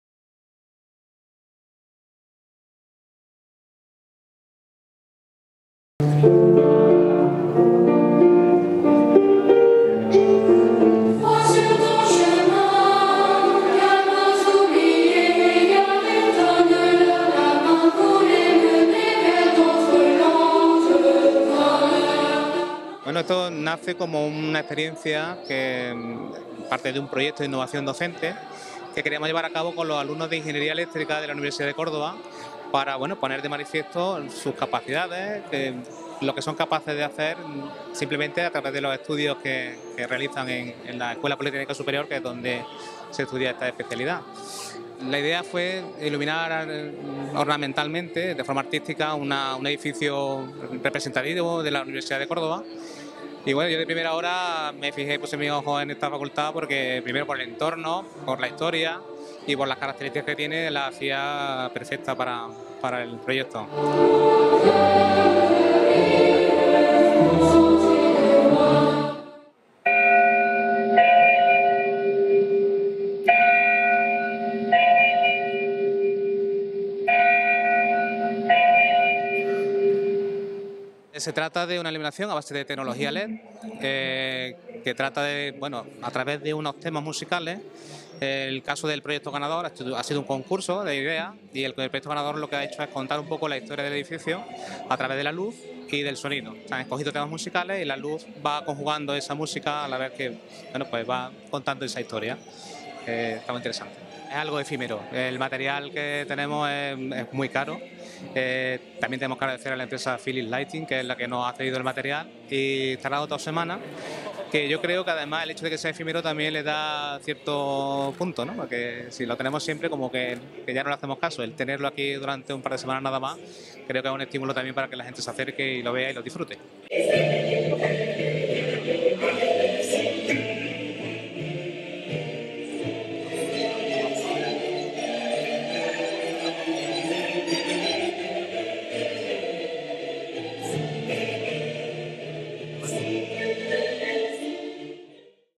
uniVERsidad. Espectáculo lumínico en la fachada Filosofía y Letras
La Universidad de Córdoba y Philips Lighting presentan un espectáculo lumínico en la fachada de la Facultad de Filosofía y Letras